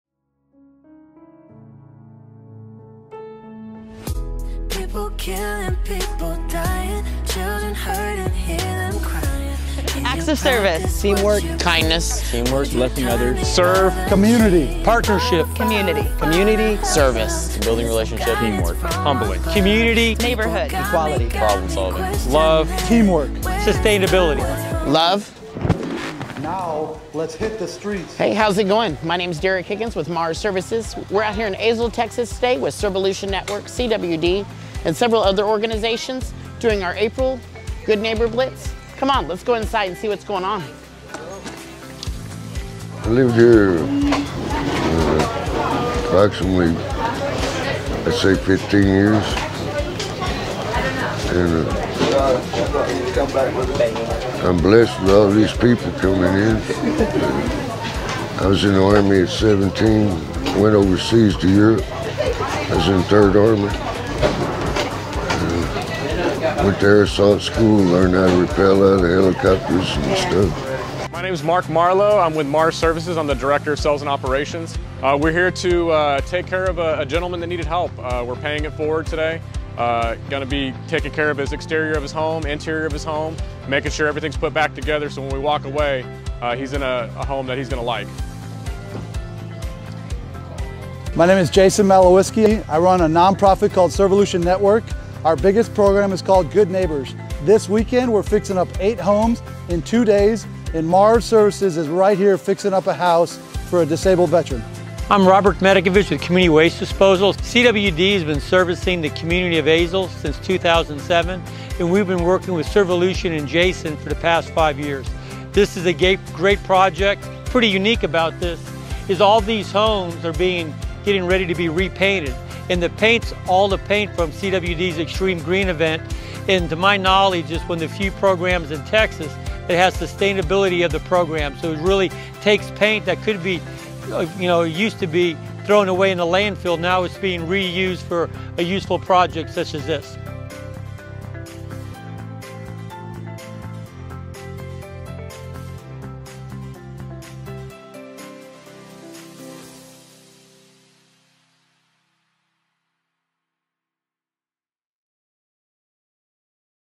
Mars Services -- April 2021 Blitz Humanitarian Video
AWESOME BLITZ RECAP VIDEO by Mars Services!!!
Humanitarian work requires being responsible, conscious of the circumstances of other people's lives, and helping them on the basis of need, without discrimination.

On house three they partnered with BME Exteriors, LLC., Guaranteed Foundation Repair, Community Waste Disposal, Rasa Floors, One Hour Air Conditioning & Heating, 4A Excavation LLC, Kingdom tree care, Prout Pro Services, Texas Premier, Silver Creek Structures LLC & Barto Electric to serve a Veteran in need.

"We at Mars Services are grateful and honored to be part of such an amazing organization that helps out so many people every year!
To get involved please reach out to Jason with Servolution Network for more information on how you can make a difference in your community."